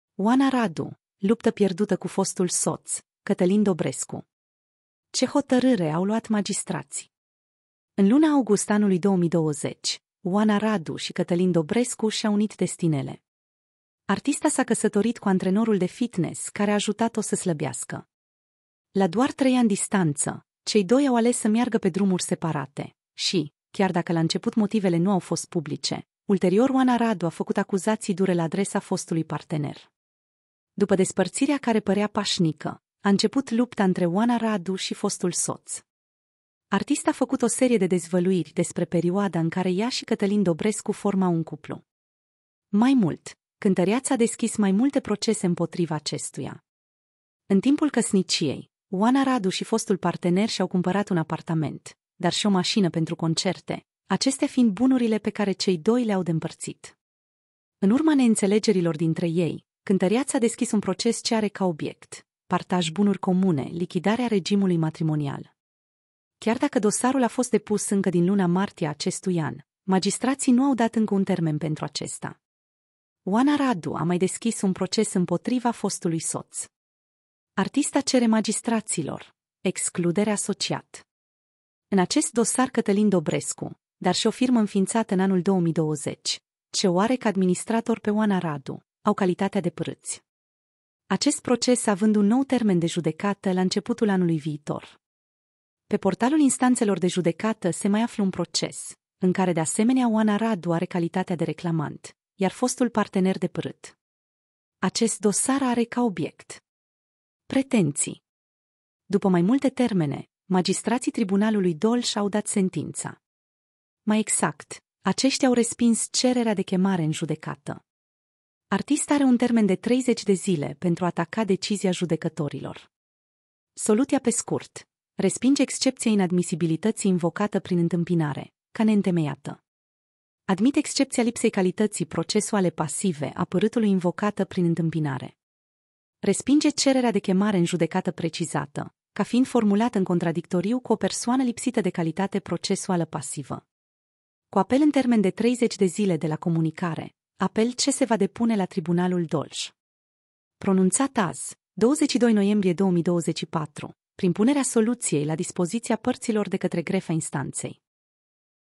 Oana Radu, luptă pierdută cu fostul soț, Cătălin Dobrescu! Ce hotărâre au luat magistrații
sursele noastre: Spynews
                             Romania tv
                             Viva
                             Ciao
                             Playtech
                             Cancan